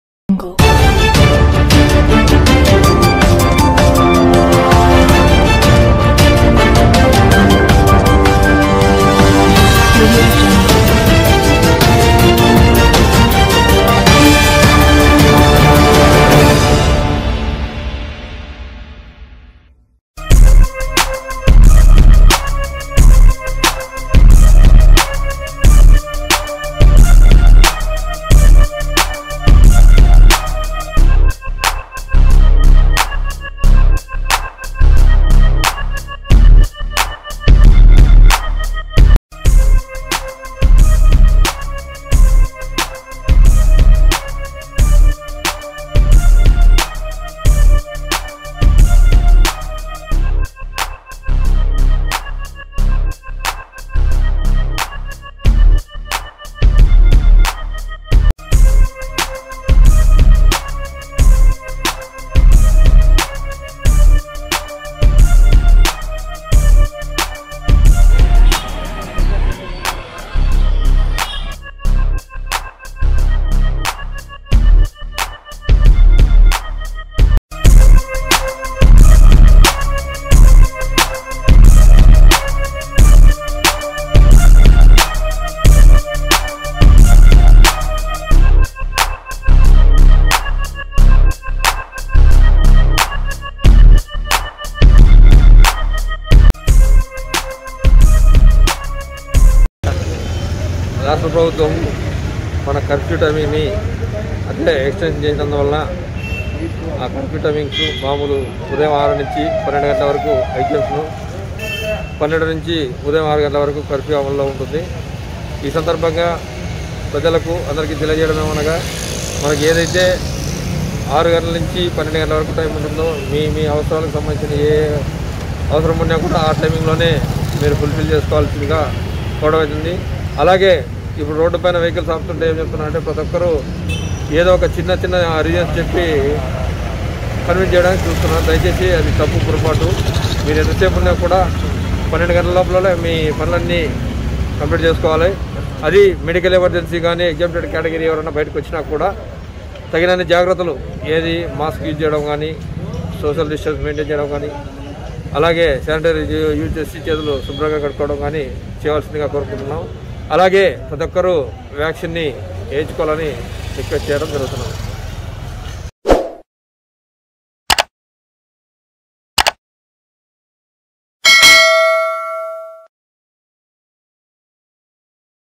నంద్యాల లో మధ్యాహ్నం 12 తర్వాత ఎవరు బయట తిరగకుడదని తెలిపిన నంద్యాల 1 టౌన్ సిఐ ఓబులేసు
నంద్యాల లో మధ్యాహ్నం 12 తర్వాత ఎవరు బయట తిరగకుడదని తెలిపిన నంద్యాల 1 టౌన్ సిఐ ఓబులేసు , మాస్క్ సానిటైజర్ కచ్చితంగా వాడాలని తెలియజేశారు.